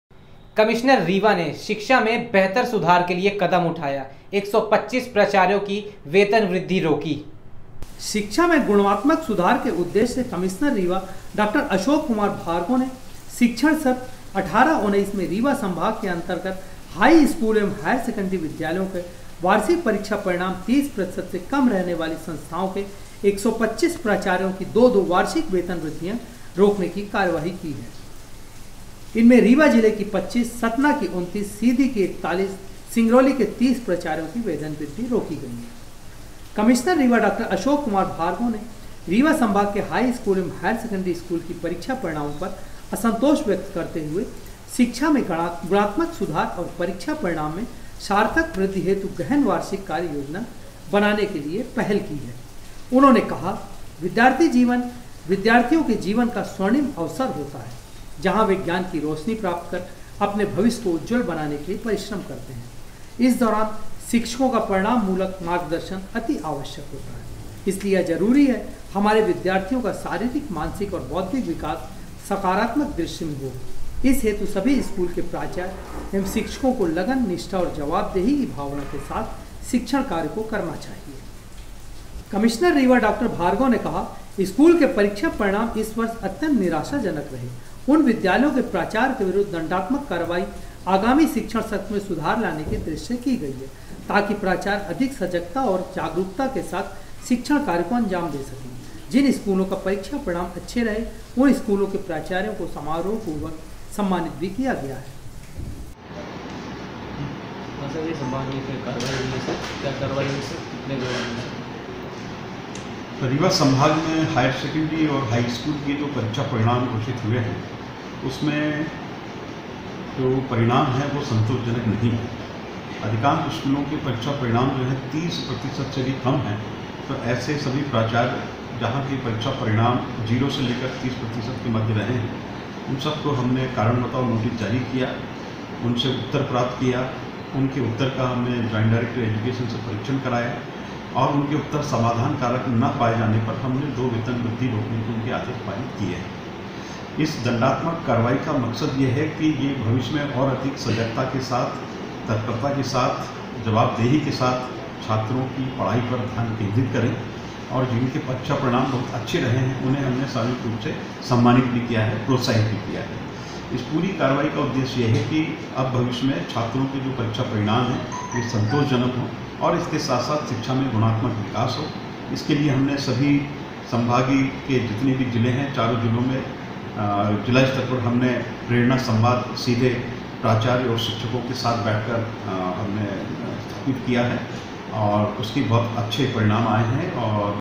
125 प्राचार्य की वेतन वृद्धि रोकी कमिश्नर रीवा ने ||NNT
125 प्राचार्य की वेतन वृद्धि रोकी कमिश्नर रीवा ने ||NNT||